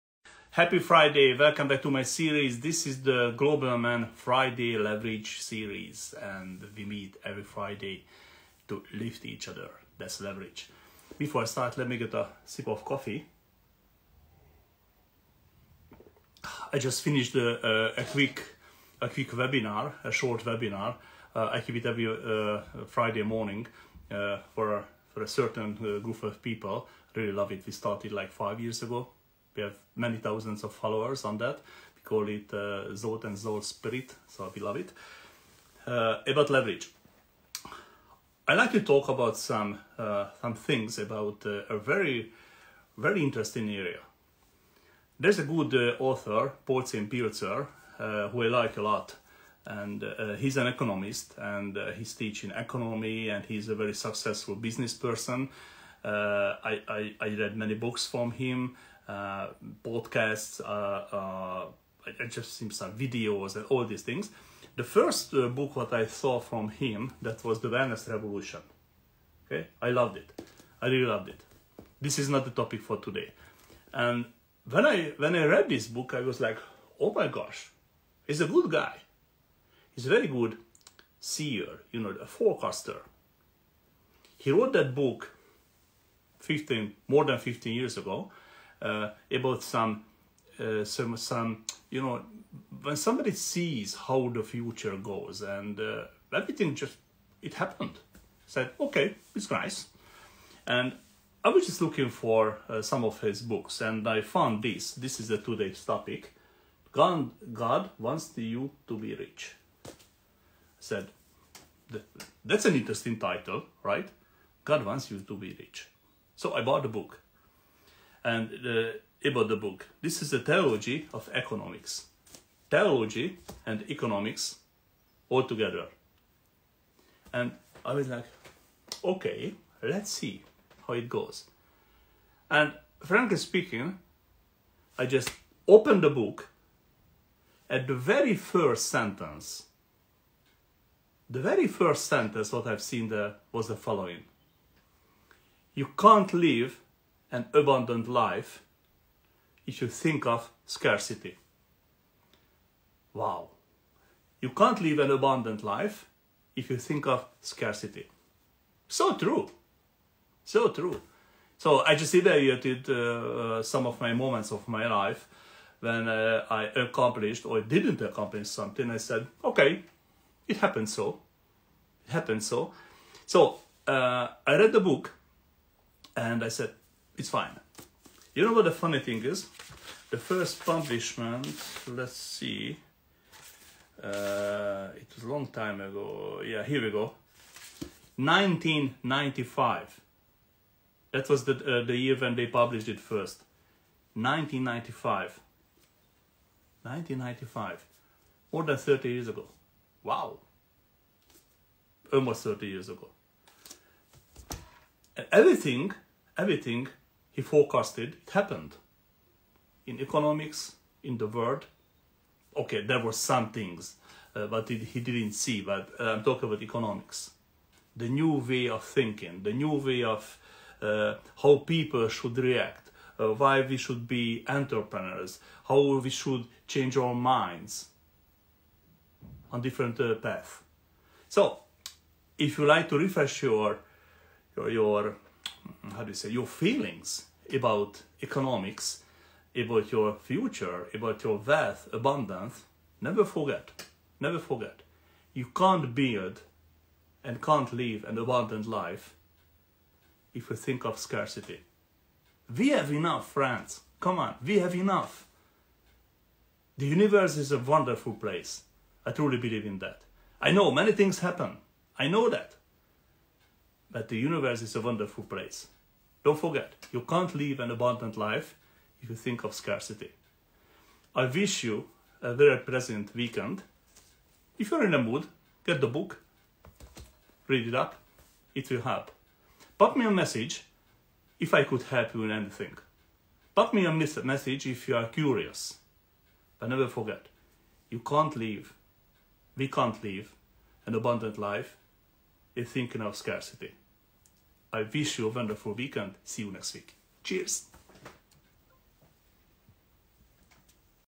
FRIDAY LEVERAGE - God wants you to be rich!
theglobalman FRIDAY LEVERAGE
God wants you to be rich by Paul Zane Pilzer
You can’t live an abundant life in thinking of scarcity!
Take this advice, so true! 😎❤️🌎